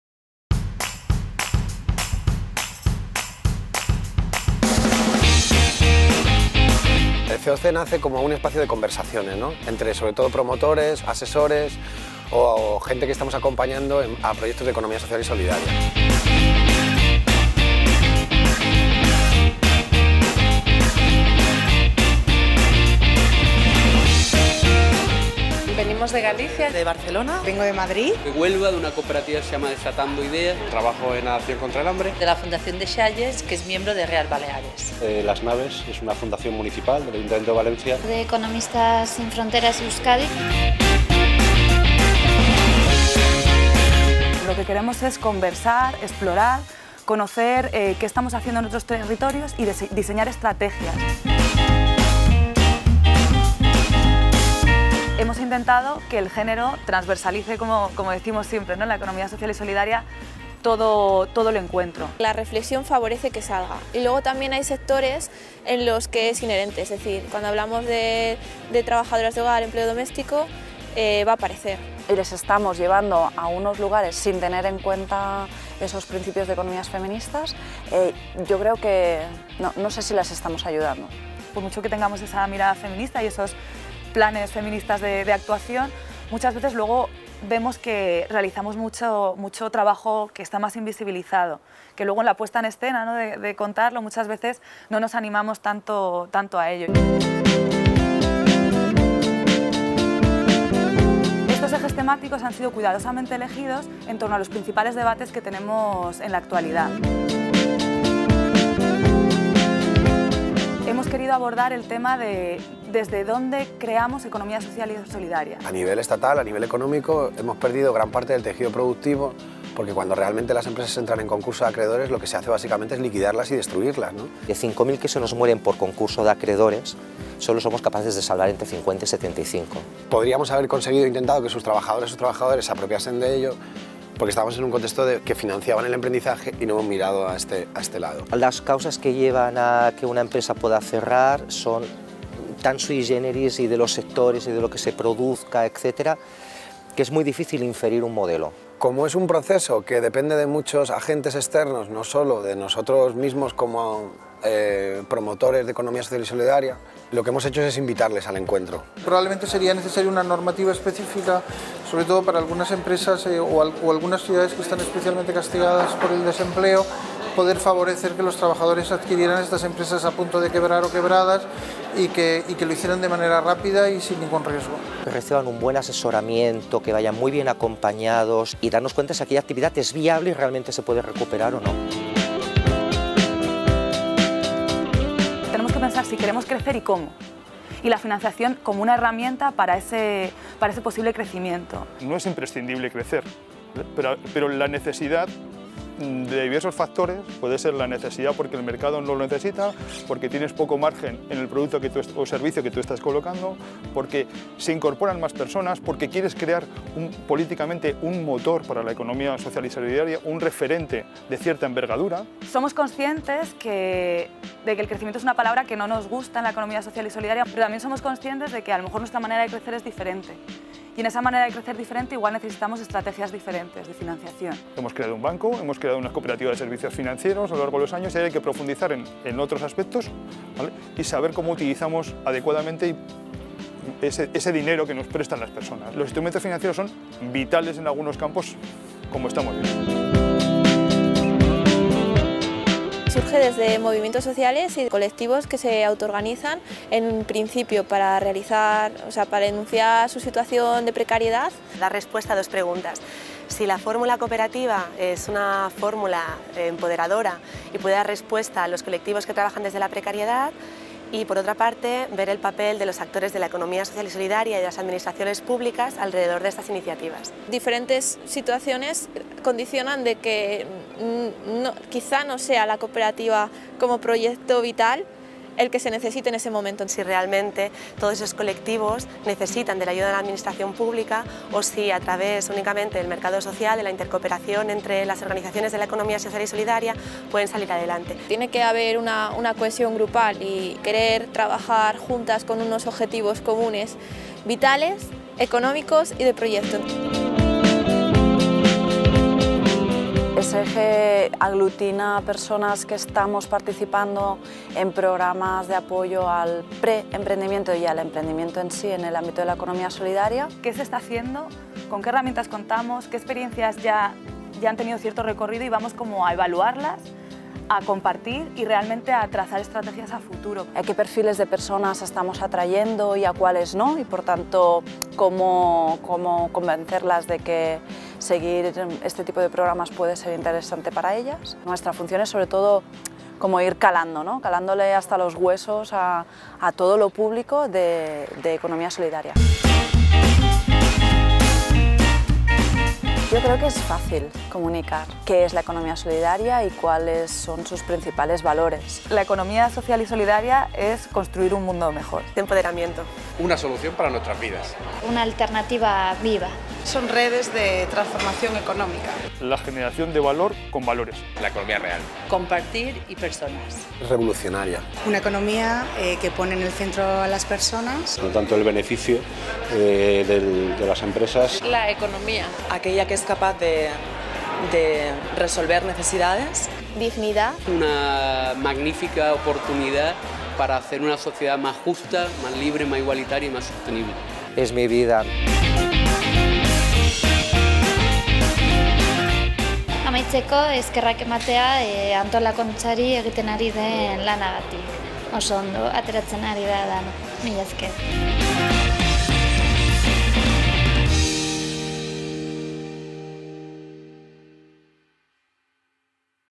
C2C CONVERSACIONES 2ª EDICIÓN MADRID, 15 Y 16 DE FEBRERO DE 2018
Os presentamos la segunda edición del C2C Conversaciones, un  foro de promoción del tejido económico social y solidario que  tuvo lugar en Madrid los días 15 y 16 de febrero de 2018.

C2C Conversaciones es un encuentro promovido por Reas Madrid y otras organizaciones de ESS que forman parte de REAS red de Redes, en un contexto de crecimiento de la visibilidad y de las medidas de apoyo a la ESS en nuestros territorios se hace más necesario que nunca pararnos a mirar cómo, con quién y para qué estamos haciendo las cosas. Una pausa en el camino para compartir e intercambiar.

C2C Conversaciones es un foro de profesionales, activistas y organizaciones que desarrollan actividades de promoción y acompañamiento de iniciativas de Economía Social y Solidaria.